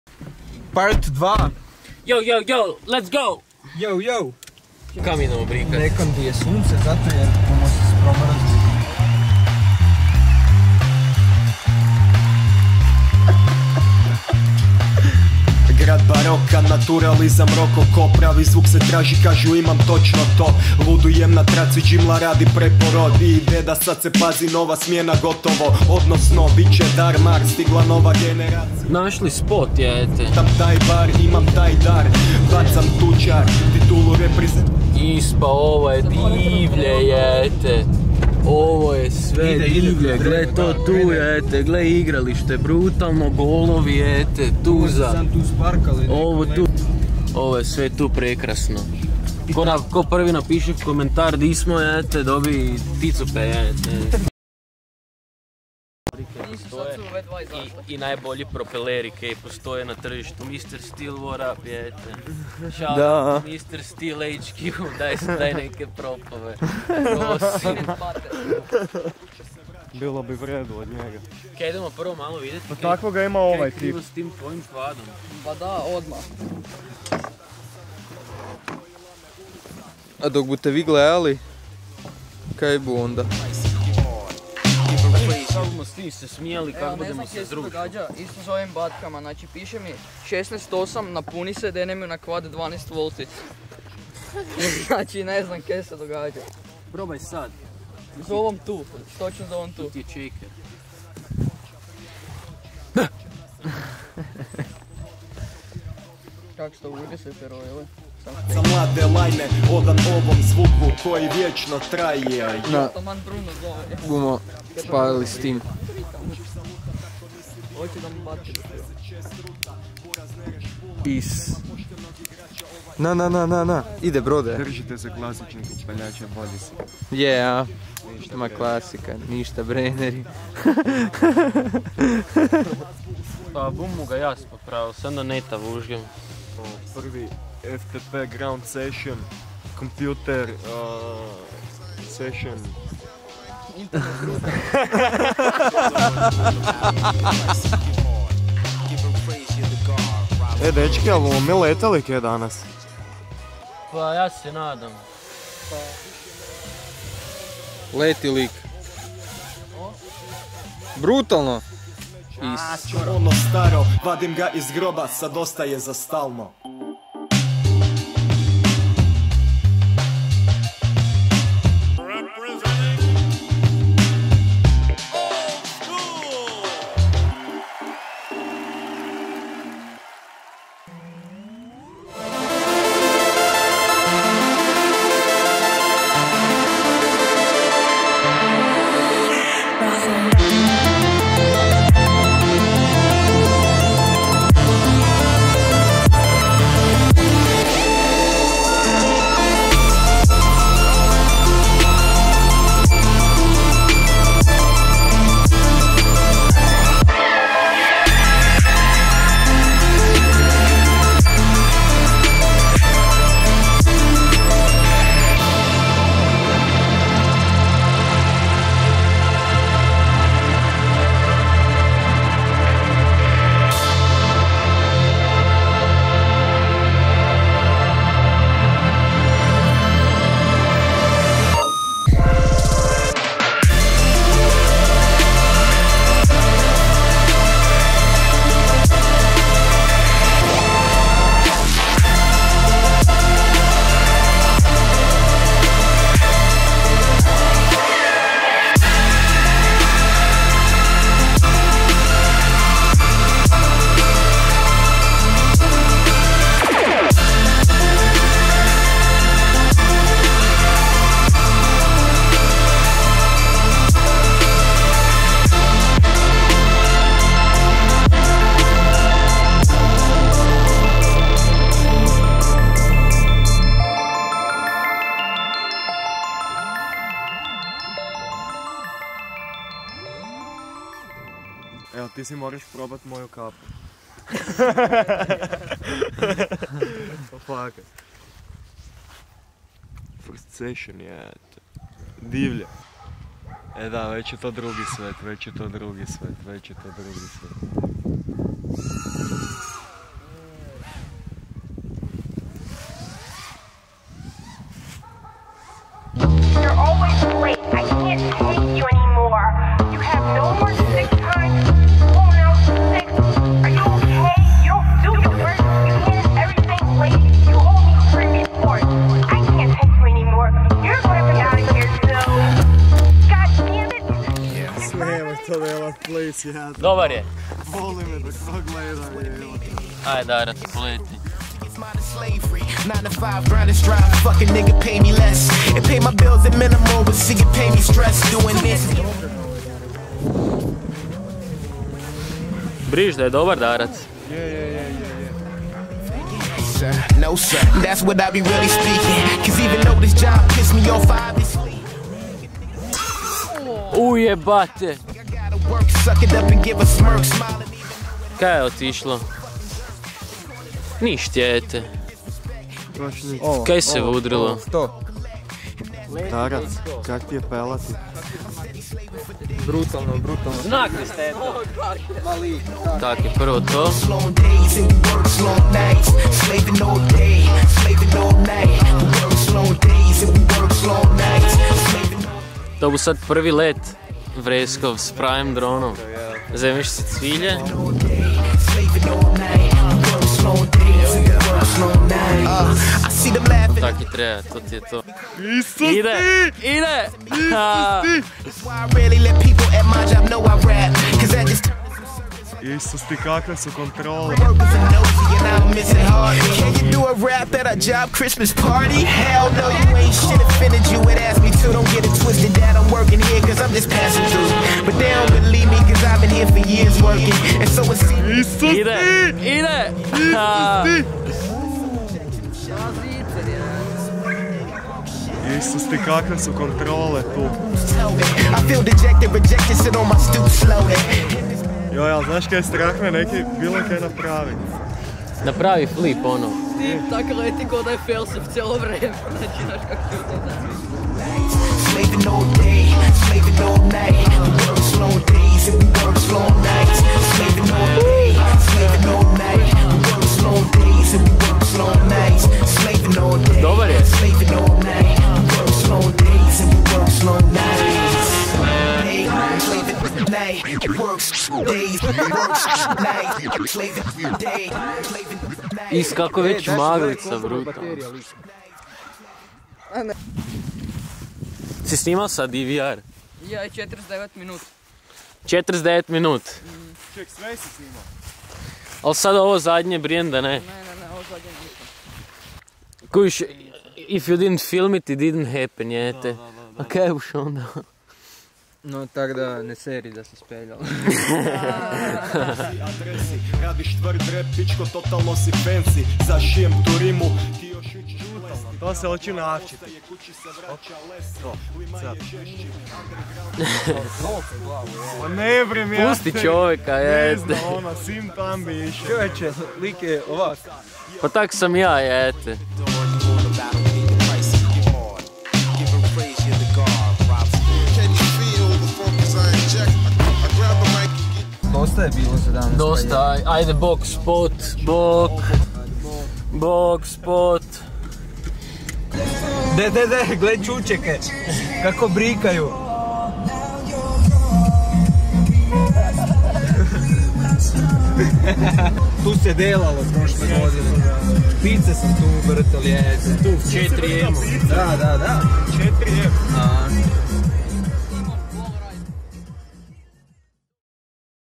Iščupali dušu iz drona - VLOG #1
U ovom vlogu nam se pridružuje Patrik Vresk iz Gossip Crew-a, ako još niste, čekirajte kanal Gossip Records i uživajte u brutalnom varaždinskom hip hop-u.